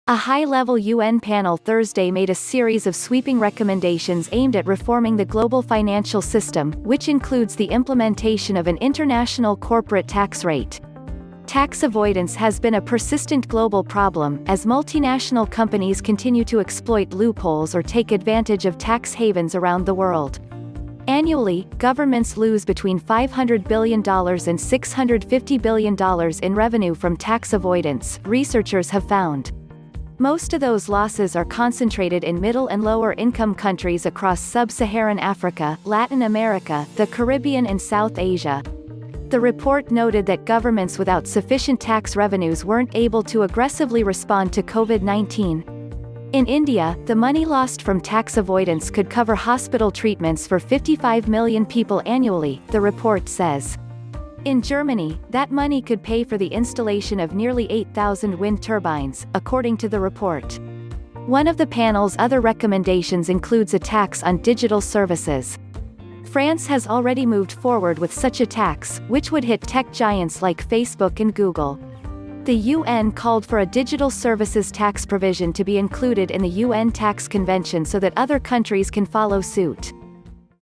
UN Panel Recommends A Global Corporate Income Tax To Cut Down On Tax
A high-level U. N. panel Thursday made a series of sweeping recommendations aimed at reforming the global financial system, which includes the implementation of an international corporate tax rate. Tax avoidance has been a persistent global problem, as multinational companies continue to exploit loopholes or take advantage of tax havens around the world. Annually, governments lose between $500 billion and $650 billion in revenue from tax avoidance, researchers have found. Most of those losses are concentrated in middle- and lower-income countries across sub-Saharan Africa, Latin America, the Caribbean and South Asia. The report noted that governments without sufficient tax revenues weren’t able to aggressively respond to Covid-19. In India, the money lost from tax avoidance could cover hospital treatments for 55 million people annually, the report says. In Germany, that money could pay for the installation of nearly 8,000 wind turbines, according to the report. One of the panel’s other recommendations includes a tax on digital services. France has already moved forward with such a tax, which would hit tech giants like Facebook and Google. The UN called for a digital services tax provision to be included in the UN Tax Convention so that other countries can follow suit.